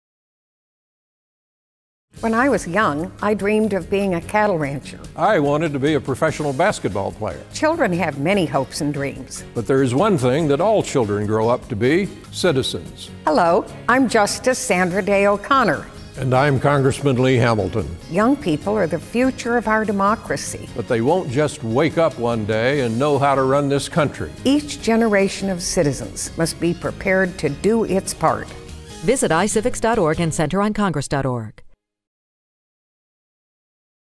Justice O'Connor's Future Citizens PSA
Justice O'Connor and former Representative Lee Hamilton recorded a public service announcement about the importance of civic education.